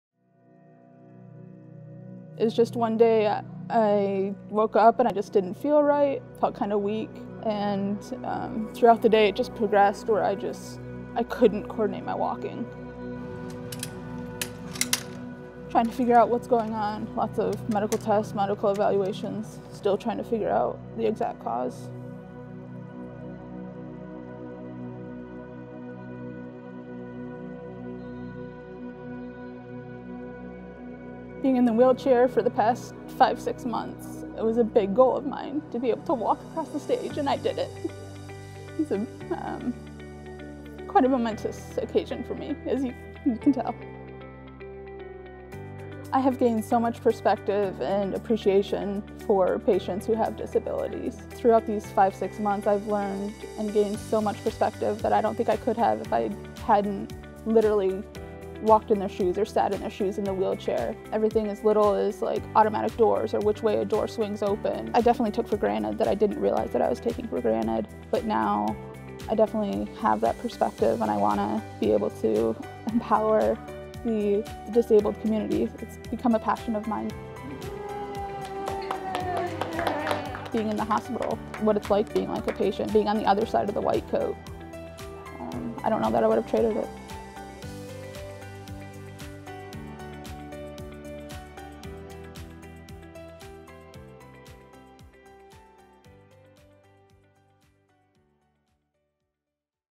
EVMS MPA Student Reaches Goal to Walk Across Graduation Stage
Helena Pett, MPA Class of 2021, has been in a wheelchair for the past five to six months with an undiagnosed condition. She made it her goal to be able to walk across the stage for commencement.